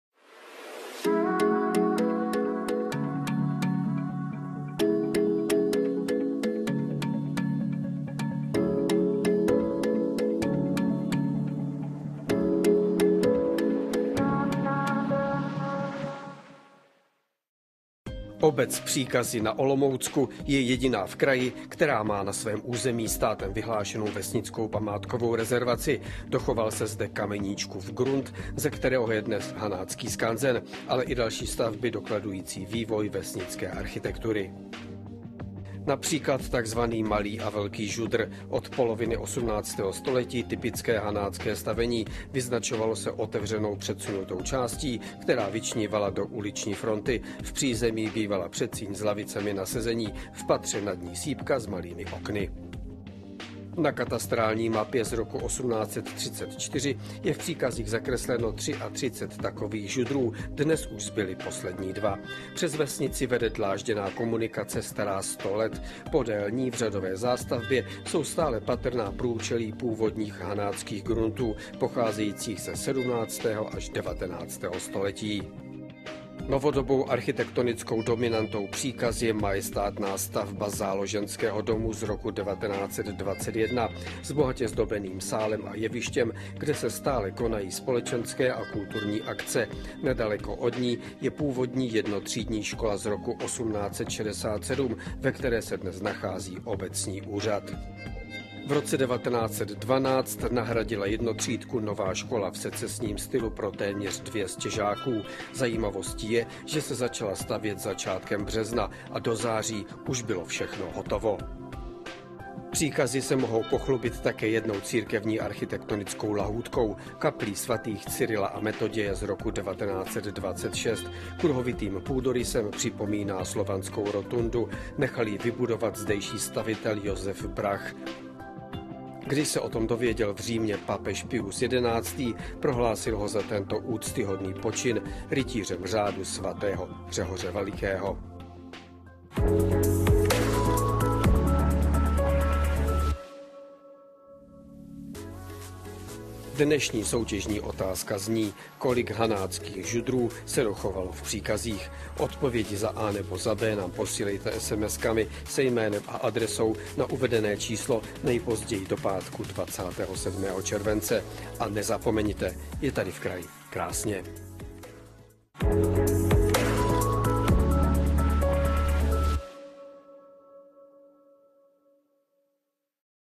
KRÁSNĚ V KRAJI - PŘÍKAZY - VESNICKÁ ARCHITEKTURA
Obec Příkazy na Olomoucku je jediná v Olomouckém kraji, která má na svém území státem vyhlášenou vesnickou památkovou rezervaci. Dochoval se zde Kameníčkův grunt, ze kterého je dnes Hanácký skanzen. Ale i další stavby dokladující vývoj vesnické architektury.